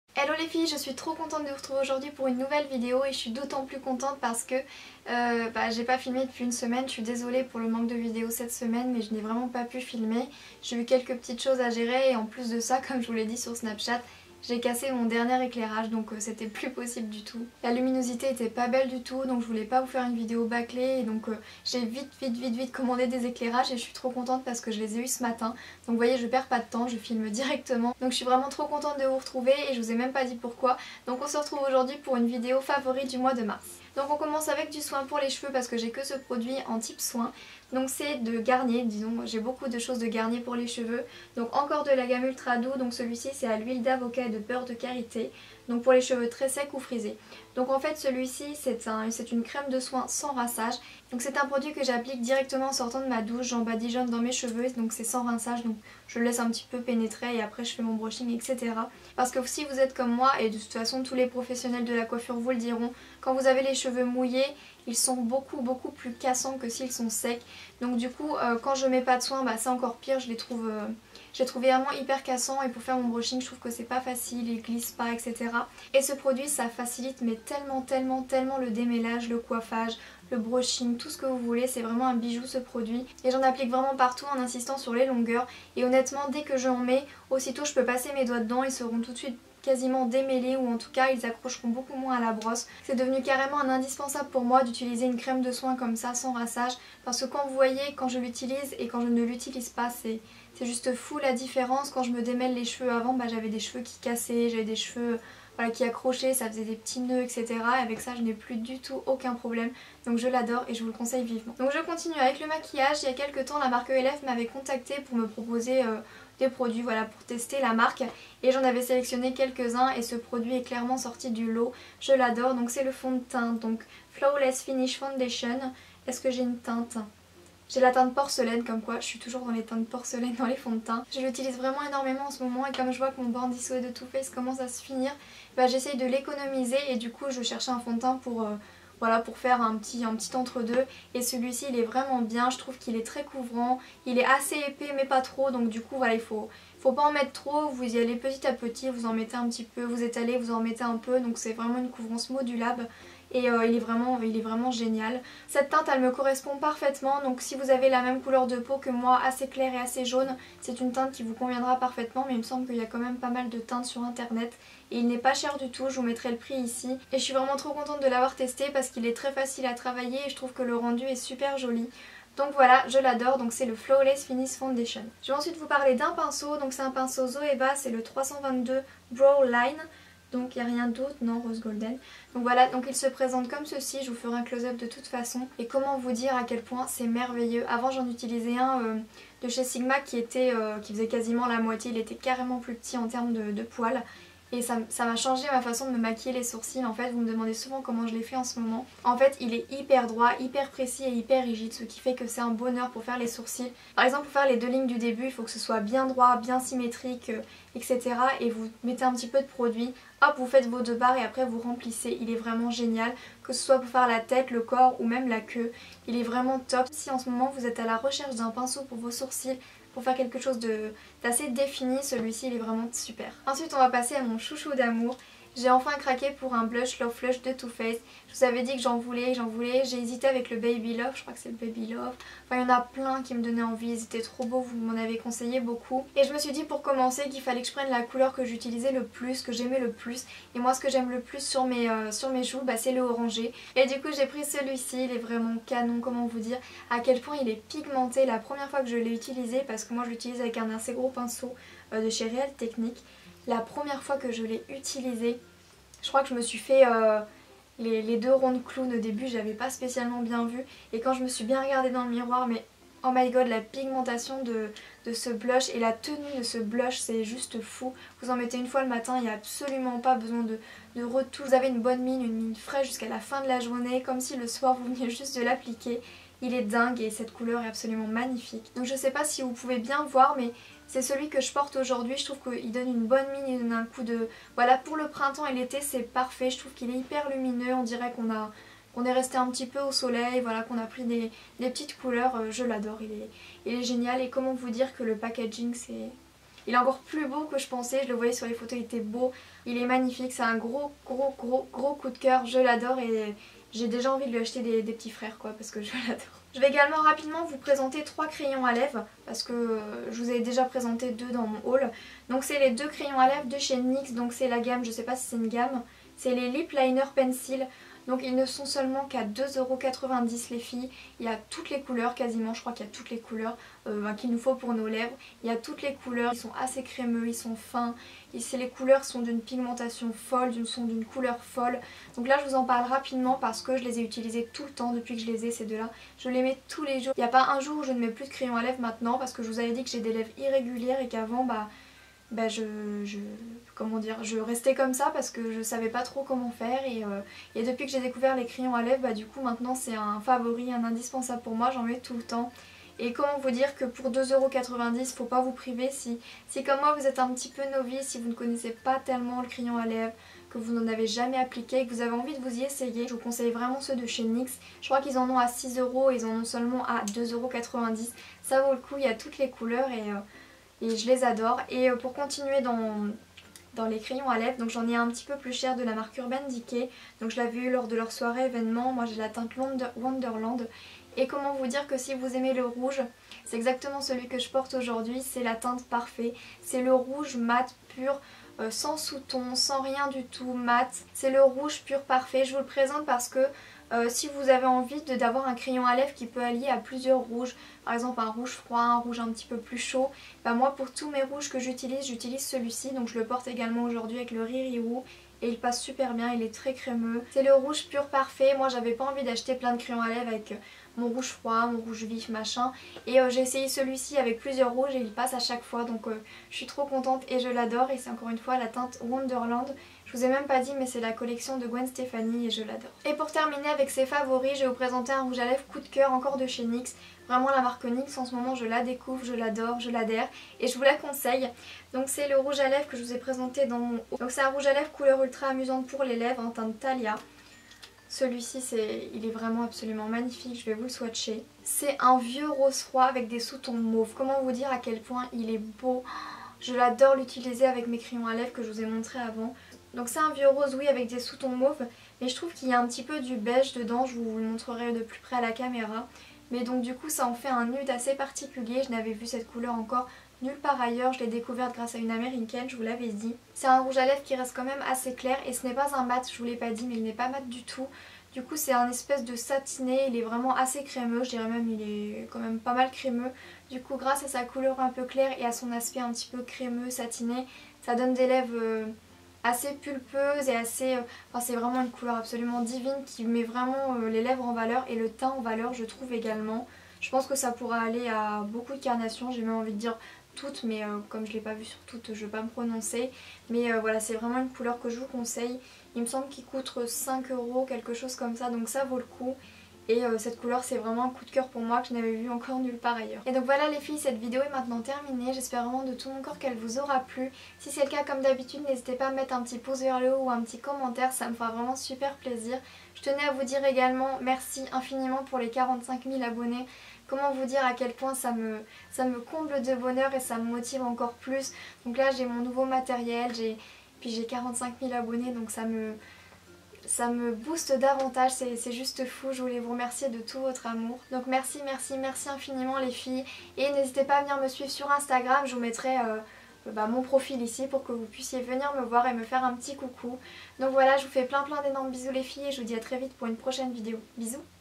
Favoris du mois de Mars ♡
♡ TOUTES LES INFORMATIONS ICI MES JOLIES ♡

▷ Hello les filles! Je suis très heureuse de vous retrouver aujourd’hui pour vous présenter une vidéo sur mes favoris du mois de Mars!
J’espère sincèrement que la vidéo vous plaira. Si c’est le cas n’hésite pas à me mettre un Like et un commentaire ça me ferai vraiment plaisir!

Beautymarieb

Beautymariebytb

 Beautymarieb

♡ Ce que je porte :
- Top : Zara
- Blazer : Zara
- Yeux : Chocolate Bon Bons Too Faced
- Vernis : Kiko

Marie ♡